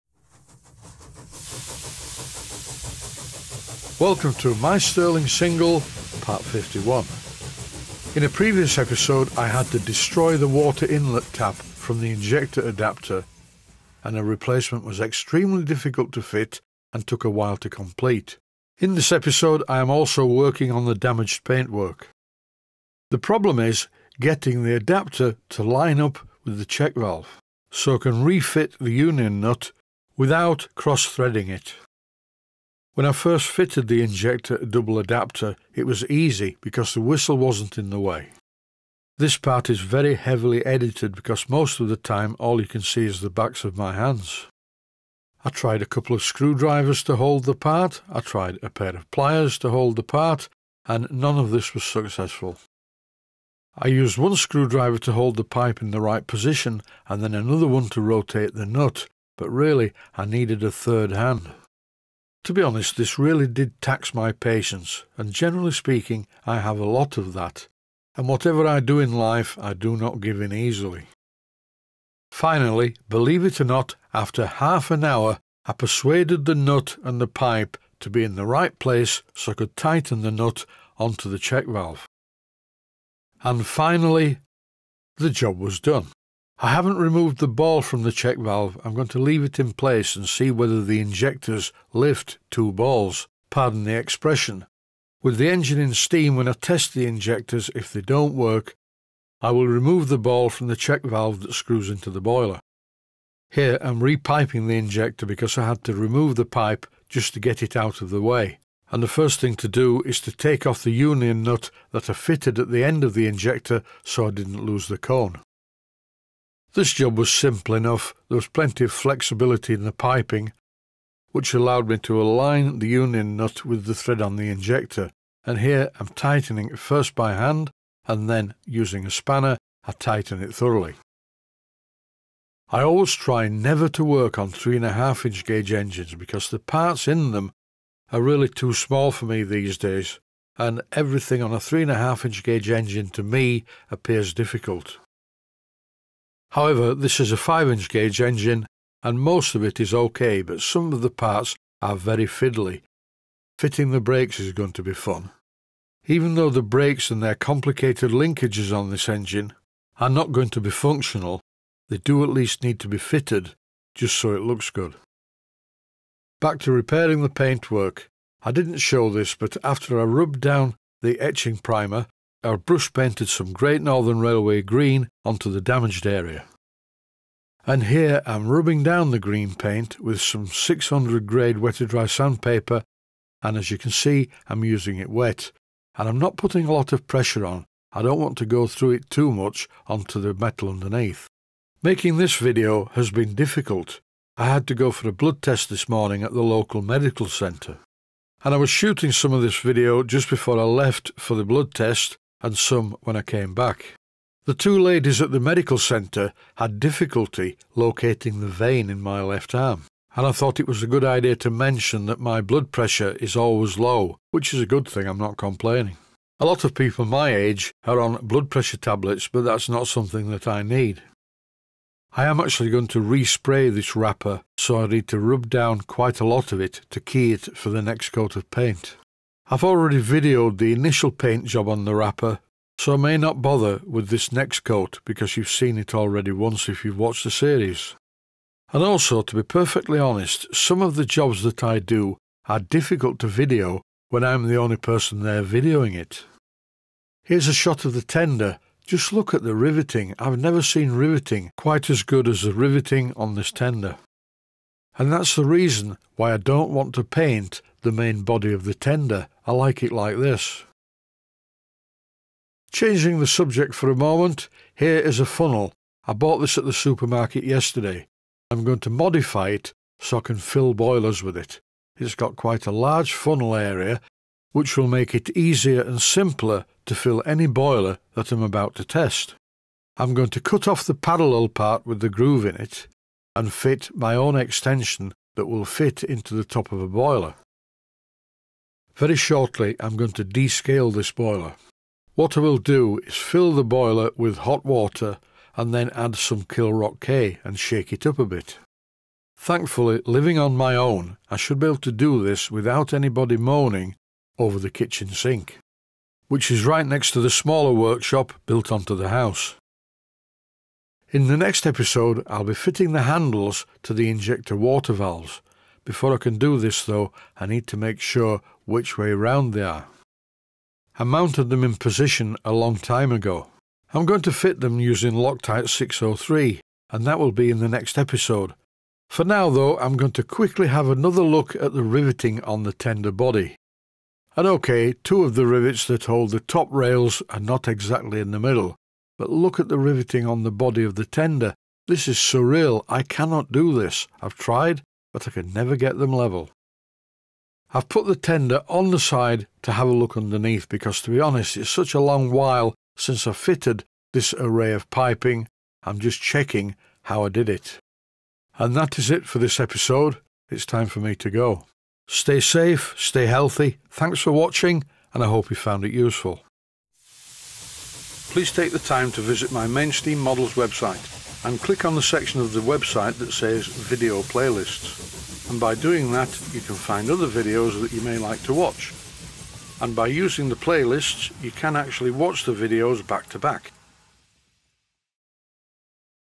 MY STIRLING SINGLE - PART #51 - FITTING THE INJECTOR INLET
My Stirling Single - Part #51 - Fitting The Injector Inlet. In a previous episode I had to destroy the water inlet tap from the injector adaptor and a replacement was extremely difficult to fit and took a while to complete. In this episode I am also working on the damaged paintwork.